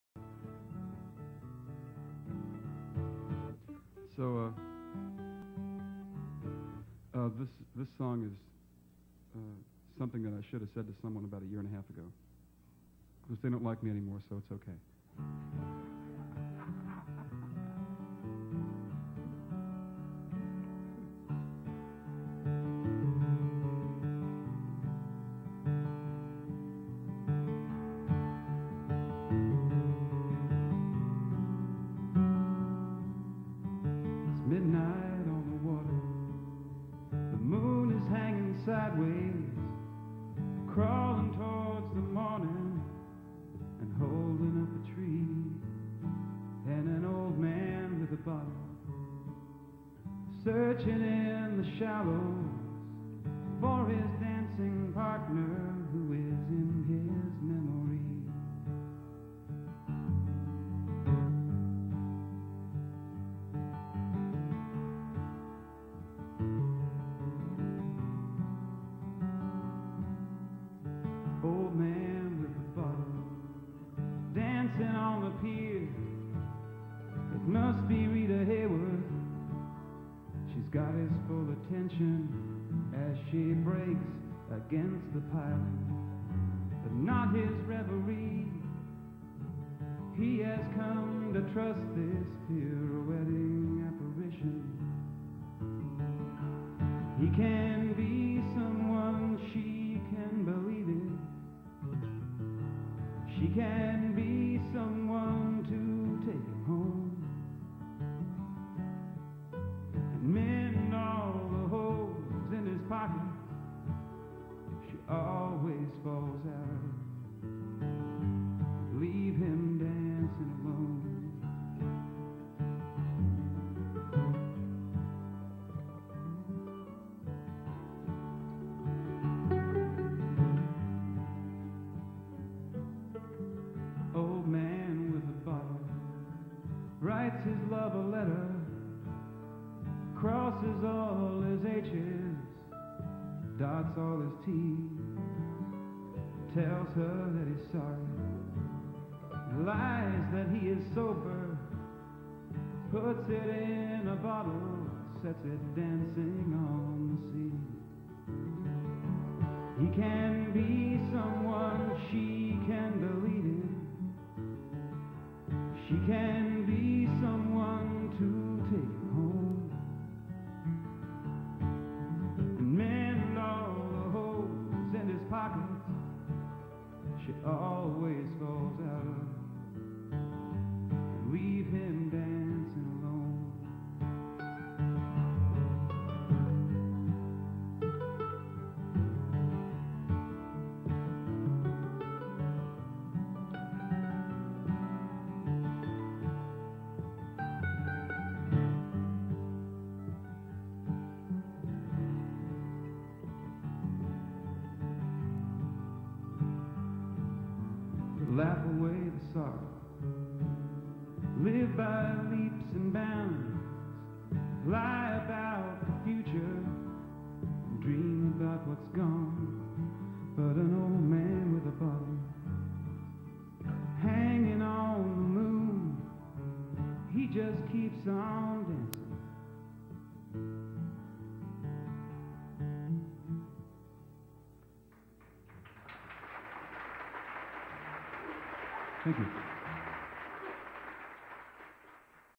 John Jennings Birchmere 1989
Another of John's listenable, relatable gems. I was shooting a series of TV shows about local performers in the early days of cable TV.